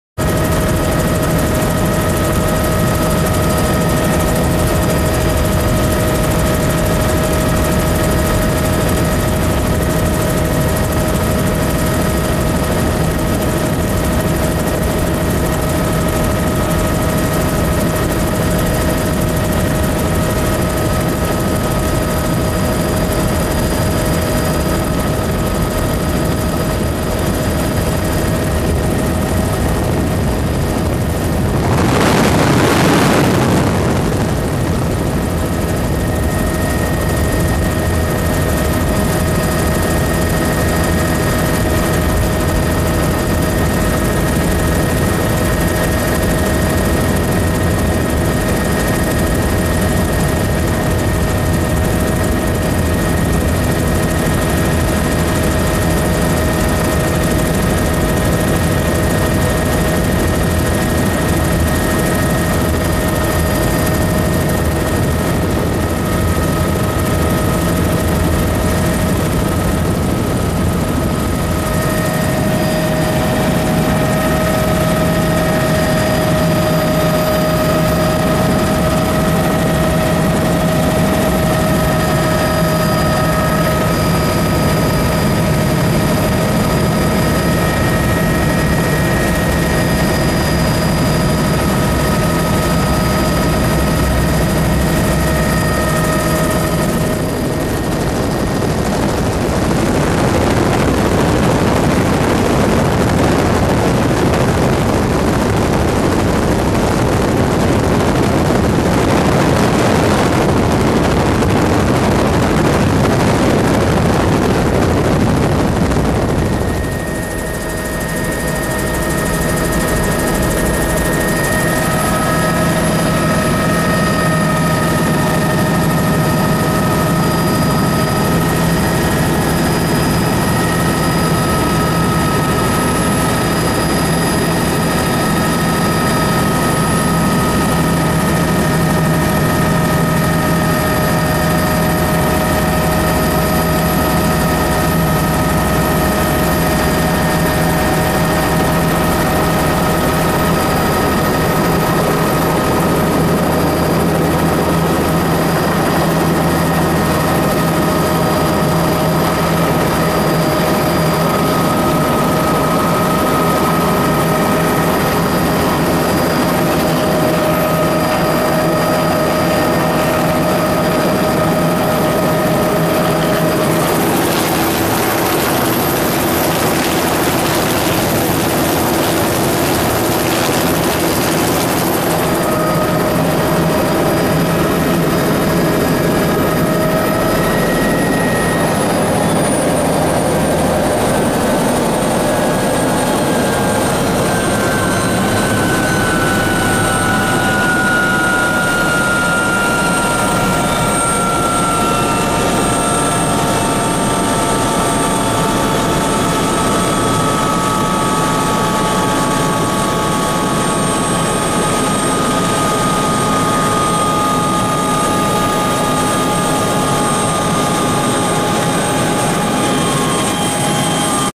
Helicopter Landing on Golfcart
Helo landing Gillespie Field (KSEE) San Diego County, City of El Cajon, CA.